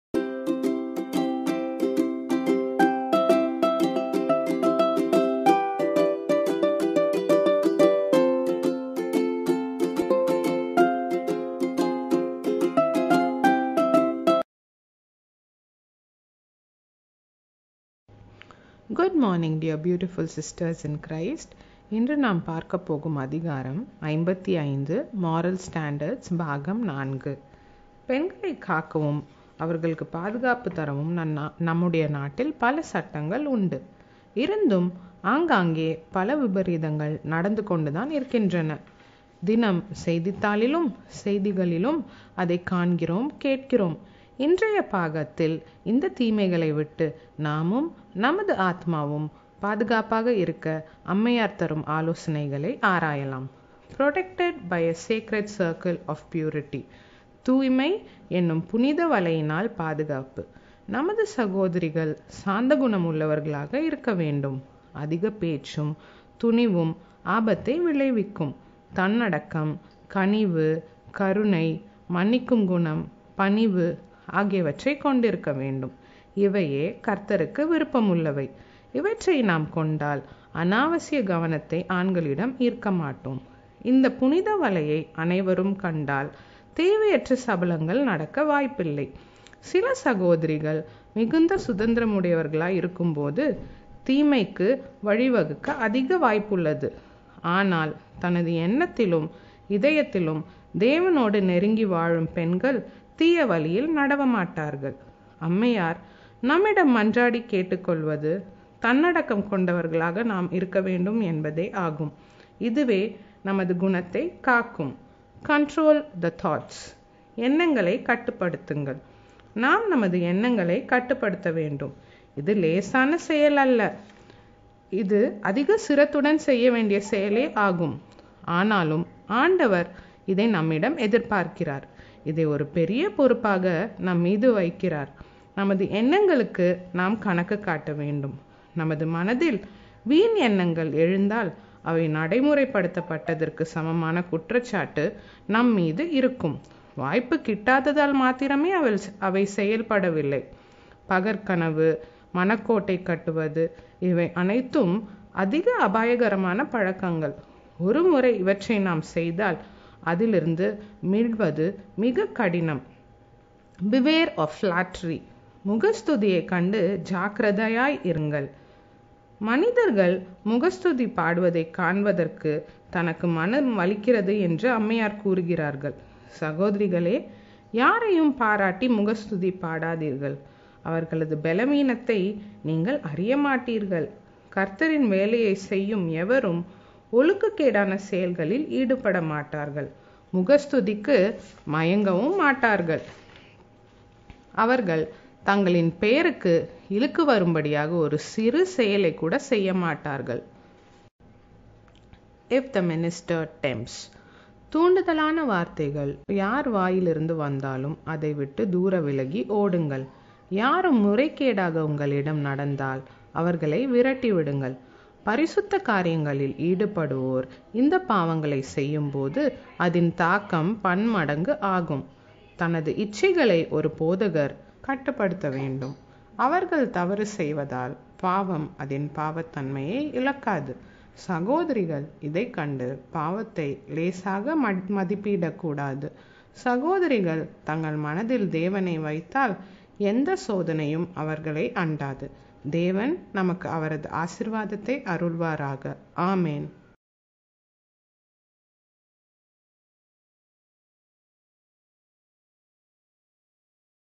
CH 55 || MORAL STANDARDS || PART 4 || THE ADVENTIST HOME
👩🏽‍🦱Women should not be forward, talkative and bold but be modest and unassuming, slow to speak.

👩🏽‍🦱 Day and night dreaming and castle building are bad and exceedingly dangerous habits.

👩🏽‍🦱Never pet and flatter poor fallible and erring men.

👩🏽‍🦱 If sisters are possessed with purity of heart, any corrupt advances can be repulsed easily.

*ஒழுக்கத்தின் படித்தரங்கள்*

👩🏽‍🦱 பெண்கள் முற்போக்குவாதிகளாக, அதிகம் பேசுகிறவர்களாக, மற்றும் அடக்கமற்றவர்களாக இராமல், நாணமும், தன்னடக்கமுள்ளவர்களும், மெதுவாகப் பேசுகிறவர்களுமாய் இருக்கவேண்டும்.

👩🏽‍🦱 இரவும் பகலும் கனவு காணுதலும், கோட்டைக் கட்டுதலும் மிகவும் அபாயகரமானப் பழக்கங்களாகும்.

👩🏽‍🦱 தவறக்கூடிய மற்றும் தவறுகிற ஆண்களுக்கு ஒருபோதும் அளவிற்கதிகமாய் சலுகைக் காட்டவோ, அவர்களை முகஸ்துதி செய்யவோ வேண்டாம்.

👩🏽‍🦱  சகோதரிகள் இருதயத்தில் தூய்மையைக் கொண்டிருந்தால், எந்த சீர்கேட்டின் முன்னேற்றத்தையும் சுலபமாக விரட்டமுடியும்.